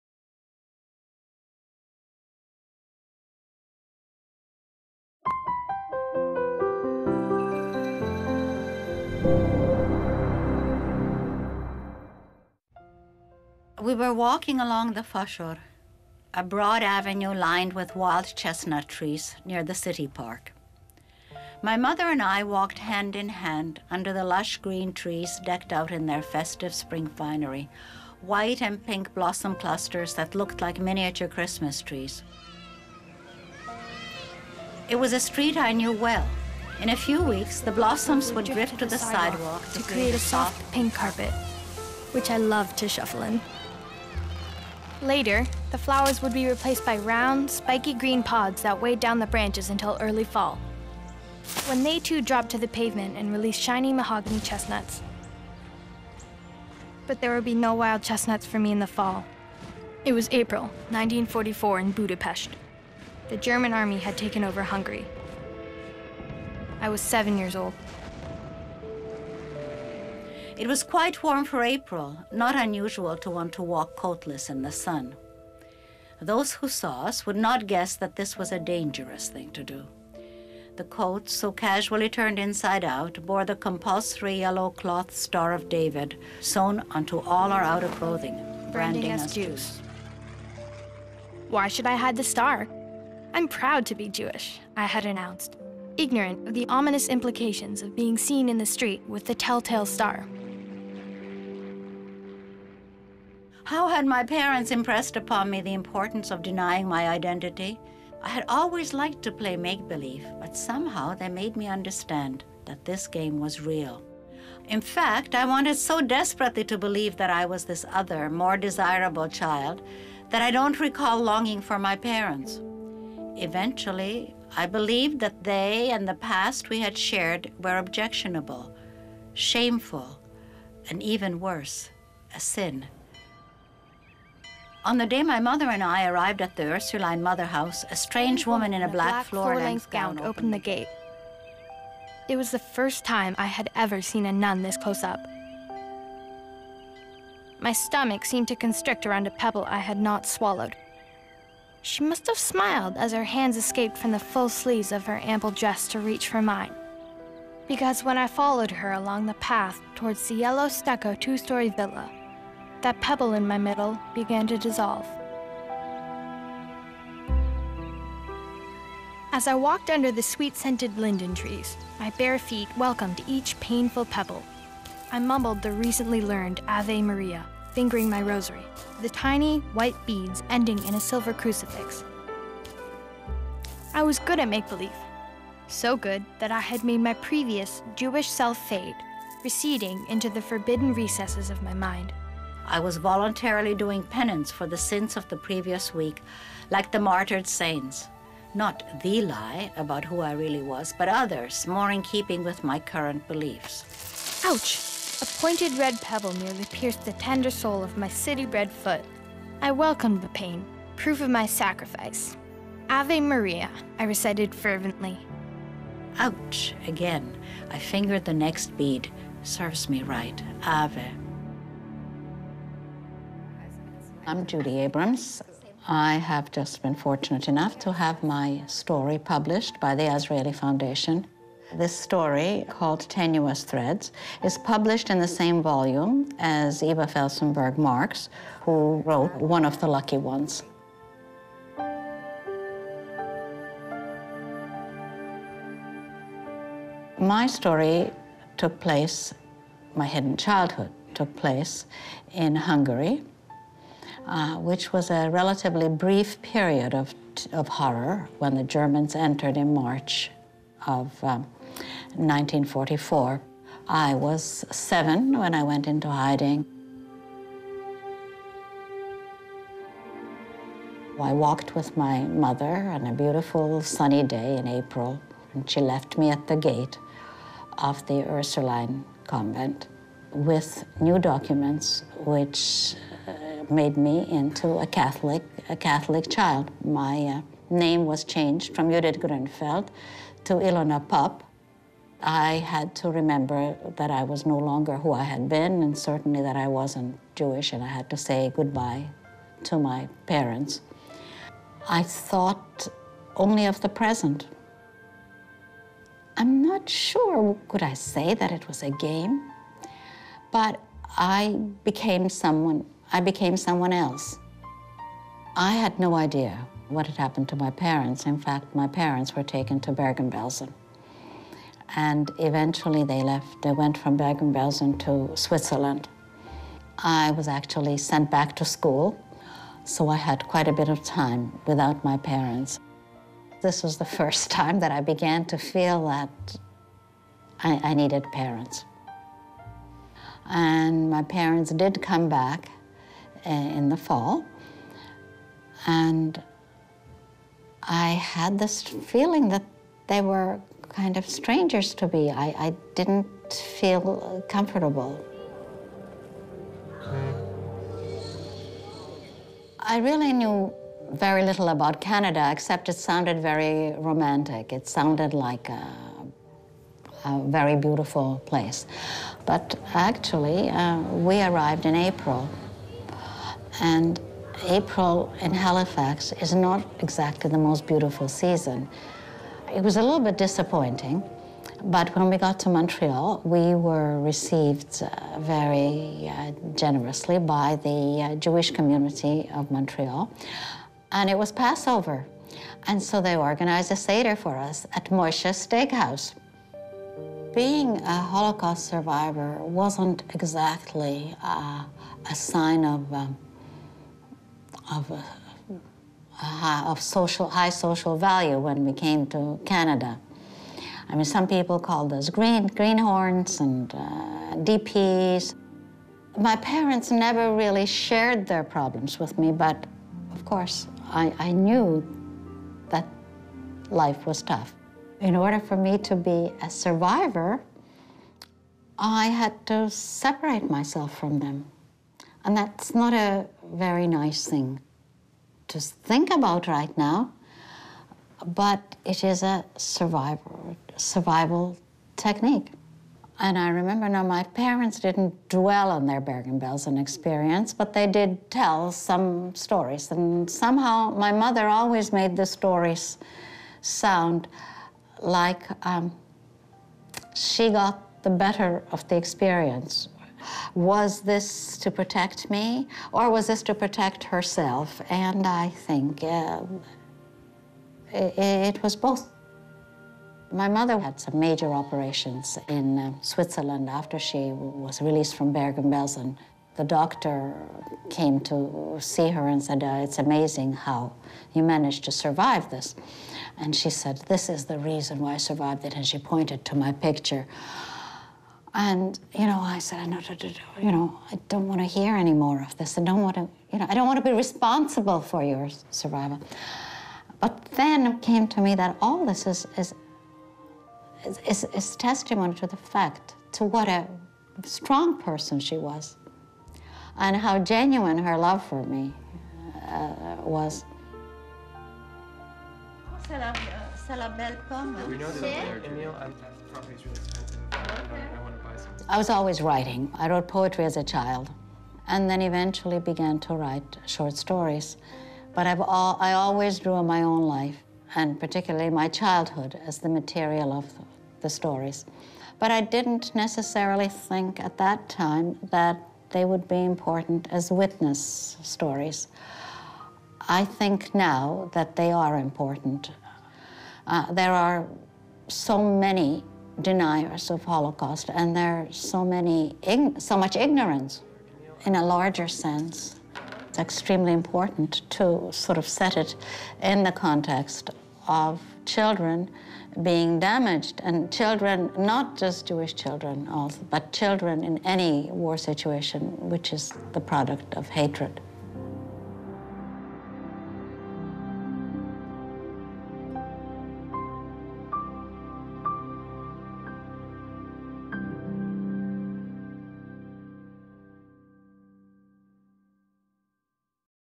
Judy Abrams - Tenuous Threads
Torn from her home and family, young Judy is forced to "pass" as a non-Jew to survive.